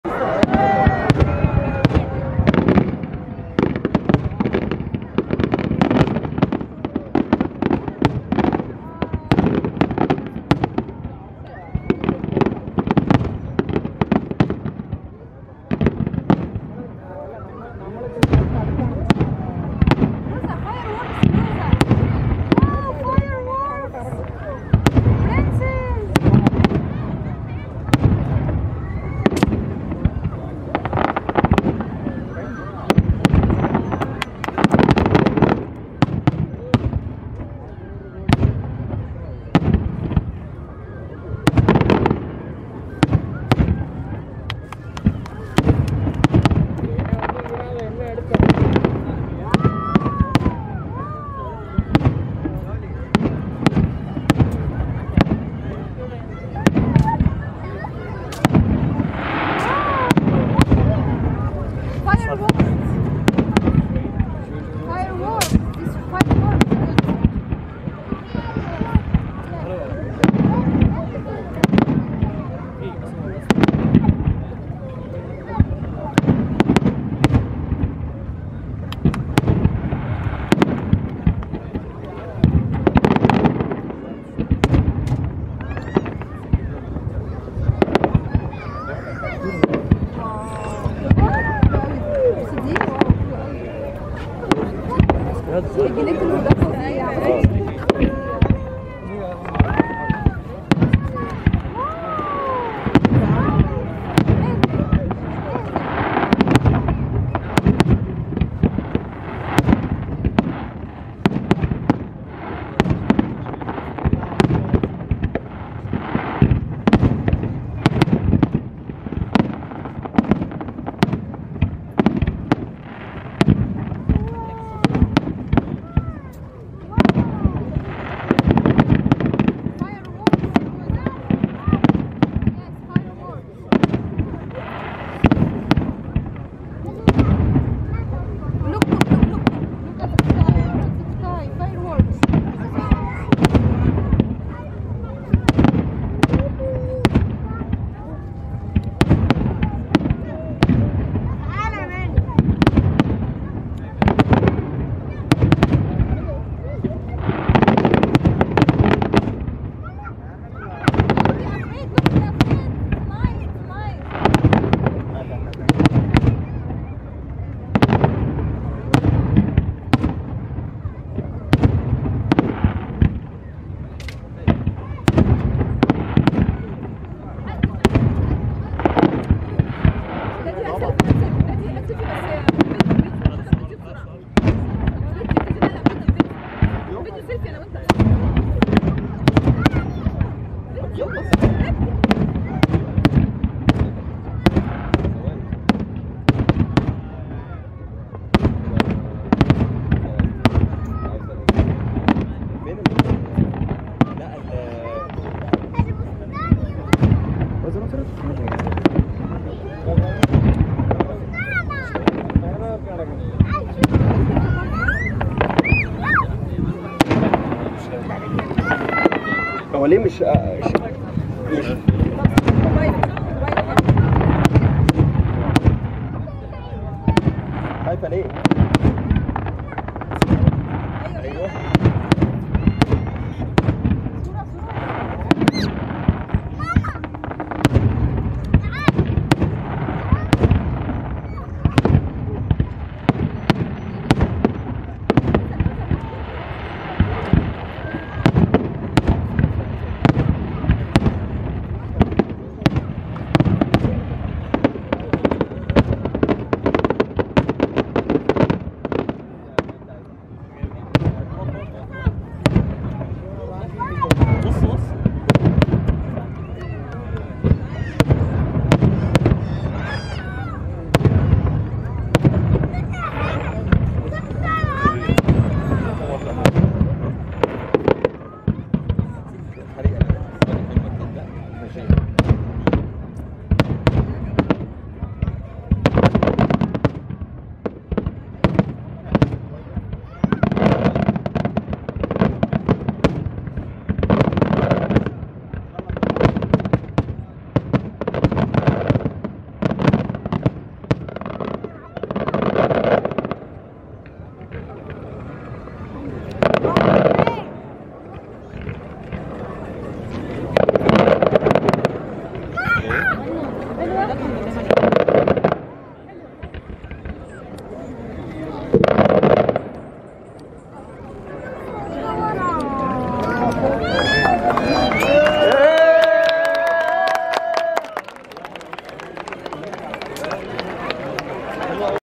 Fireworks at Khobar - Eid ul Fitr 2022- Eid Celebration in Saudi Arabia
Firework during Eid ul fitr festival at Khobar Saudia Arabia 2022/ Eid Celebration in Saudi Arabia